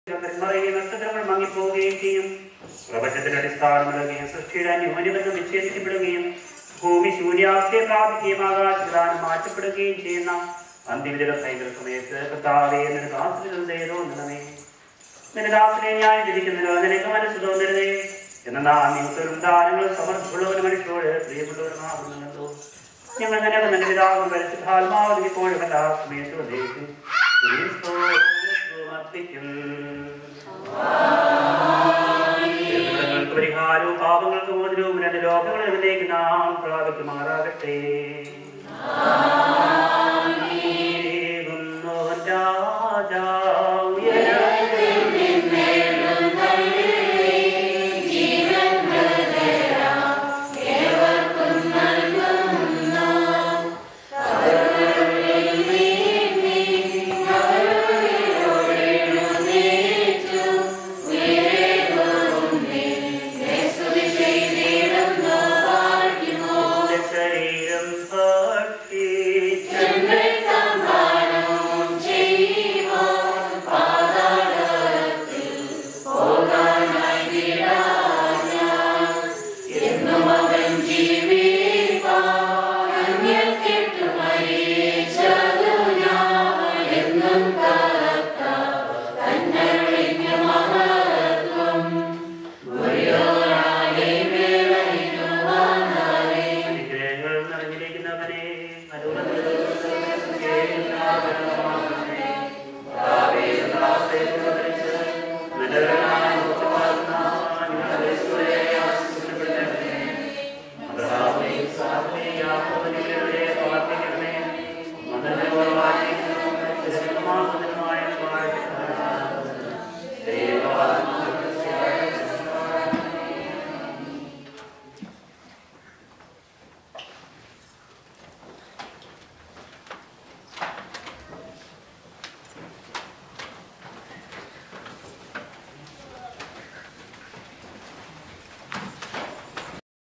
28 April 2013 - IV Sunday of Resurrection (Qyamto) - 7
IV Sunday after Qyomto
Comm. of Bishop Isaac Mar Yoohanon

Celebrant: Rev. Fr. Mathew Perumbillikunnel

Safro (Matins):
Mt 28:16-20

Thuyoobo (Prepatory)
Ex 34:10-17
Job 42:7-17
Is 49:8-13

- Holy Qurbono -

1st Reading:
Acts10:34-43

Paulos Sleeho Reading:
2Cor 5:17-21

Evangelion (Gospel):
Luke 24:36-49